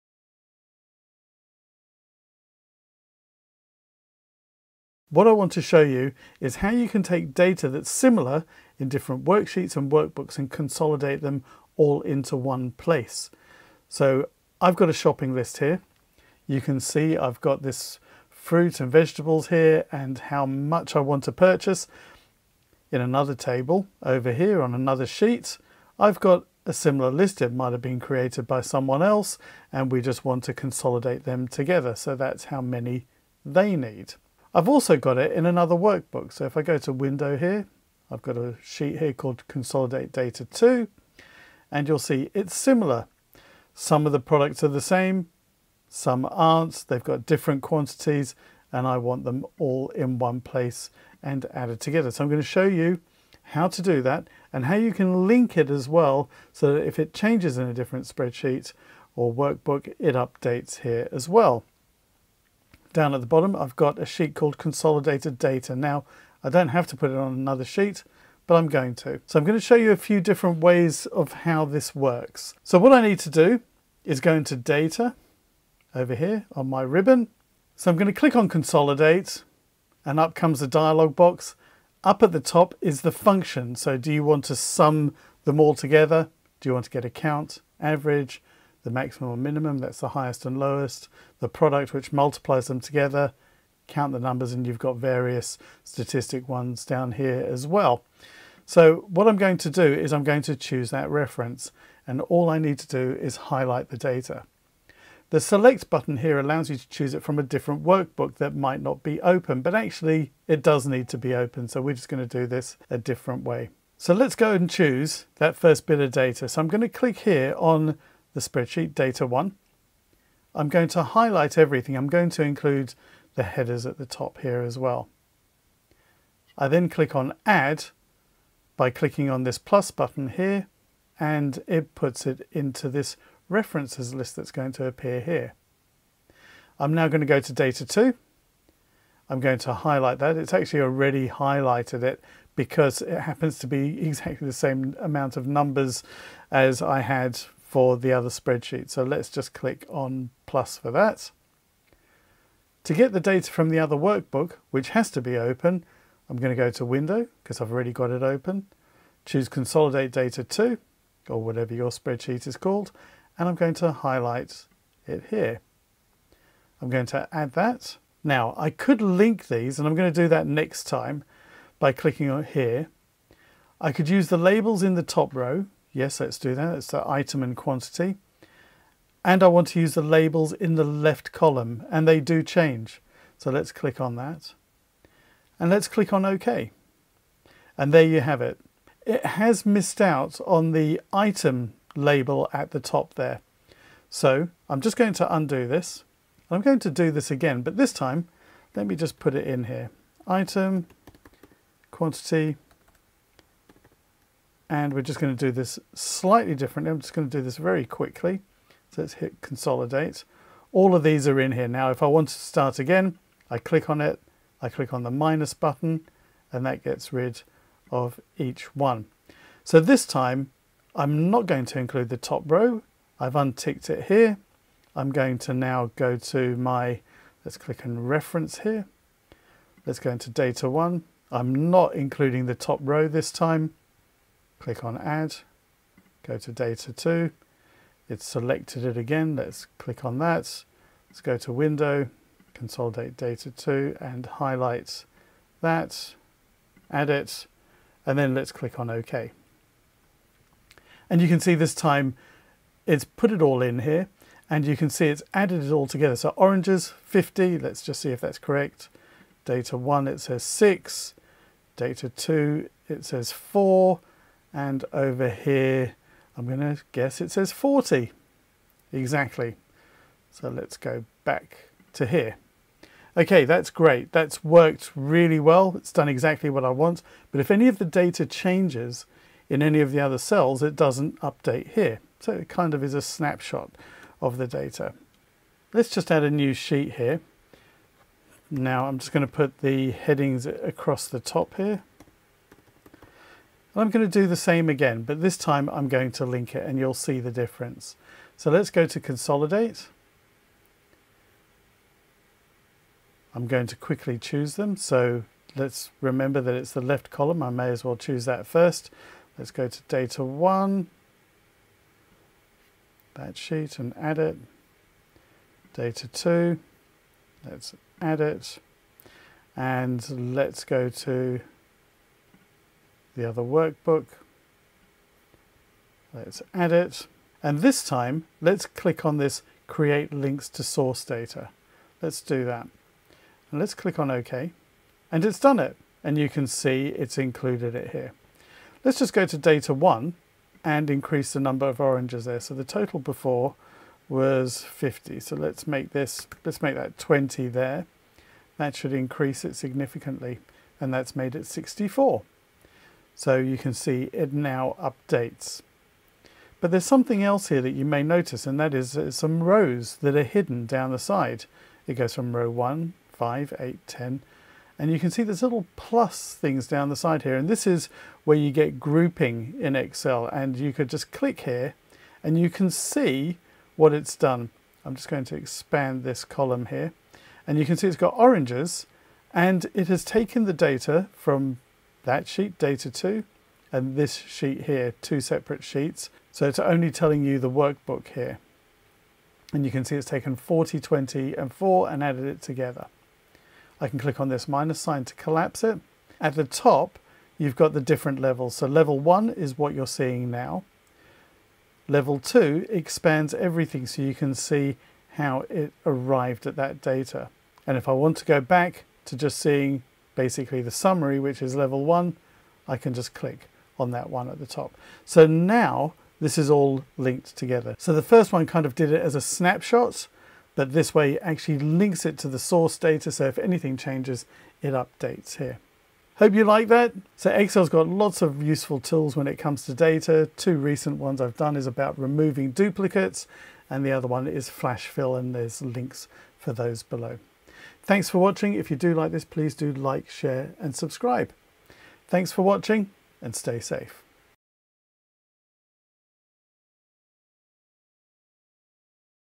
Excel: How to Use Consolidate
If you have data spread out over more than one workbook or worksheet you might want to combine it into one. This could be from different people entering data into different spreadsheets or from surveys. Excel has a useful feature called consolidate data that easily allows you to gather this information into one place.

If you want to see more tutorials on Excel or other programs then check out my YouTube channel or visit my